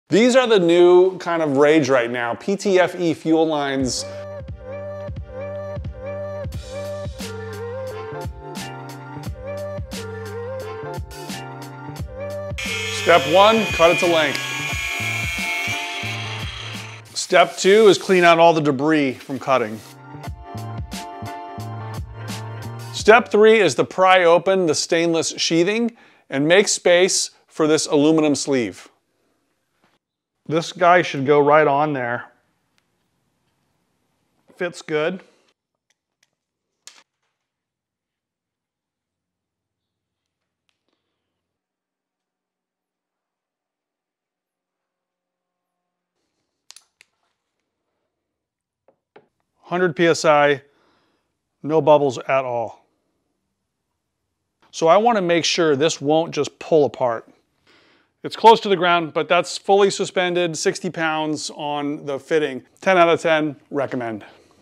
Ethanol Proof Your Fuel Lines with this PTFE Fuel Kit from Evil Energy
Check out these Ethanol proof PTFE fuel lines & fittings from Evil Energy.  They consist of a semi rigid teflon liner which is protected with stainless and fabric sheating.  Say no more to deterioriated rubber fuel lines that get hard and crack.  These should last at least 10 years if not forever.